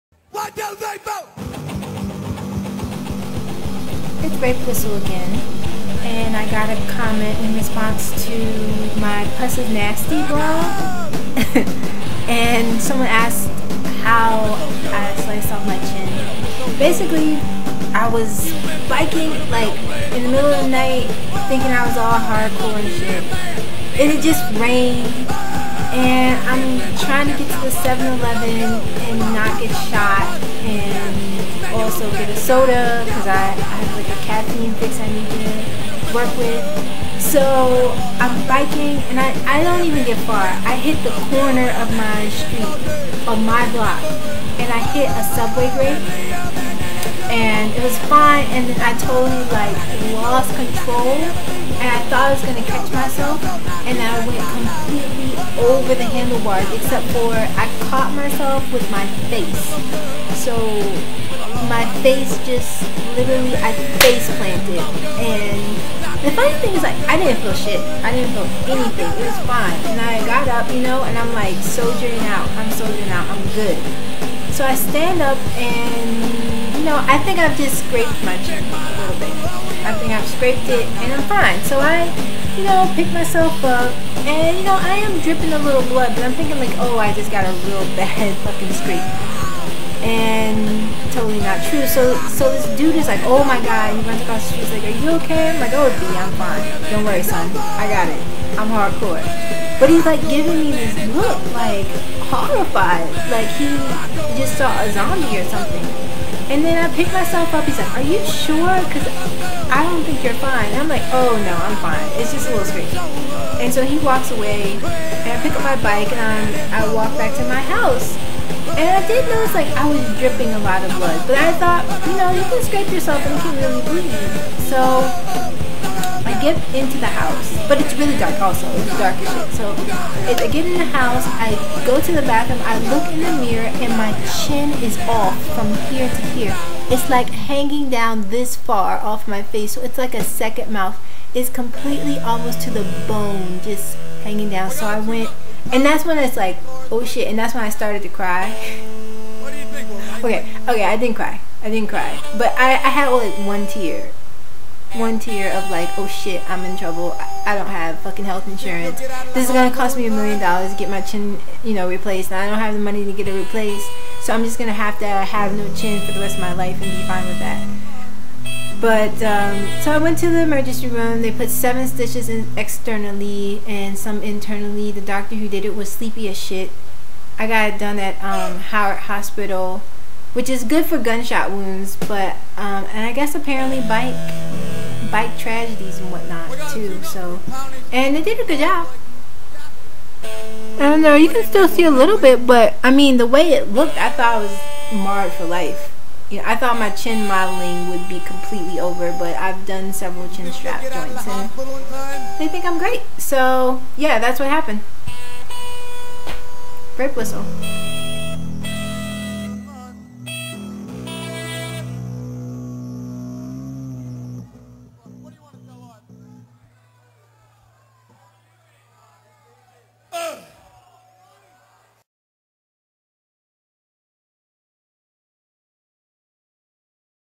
Rape Whistle -Afropunk Vlog Bike accident!
Retelling of the tragic day I lost my chin on the block